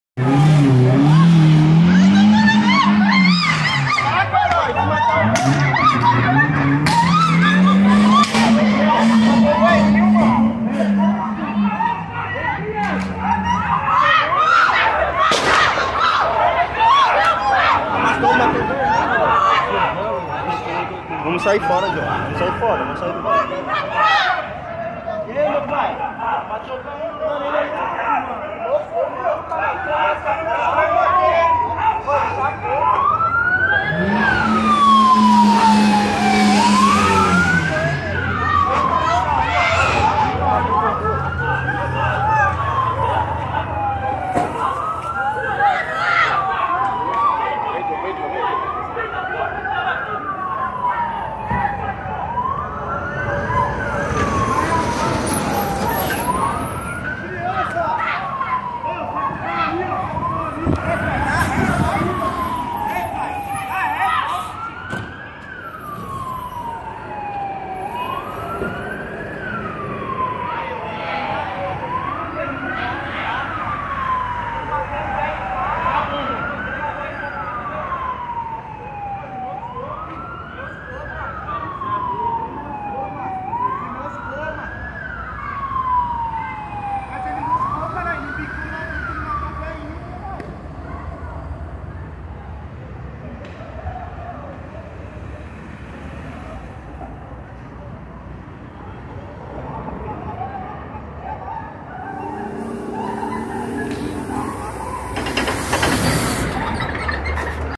GCM reage contra invasão
Agentes da GCM-SP foram vítimas de uma tentativa de invasão de um conjunto habitacional localizado na Av. Gastão Vidigal, zona oeste de São Paulo.
Invasor jogou o veículo para cima dos Policiais, atropelaram o representante dos mesmos, onde este foi arrastado e teve várias fraturas, e diante da injusta agressão, o mesmo foi alvejado e entrou em óbito no local.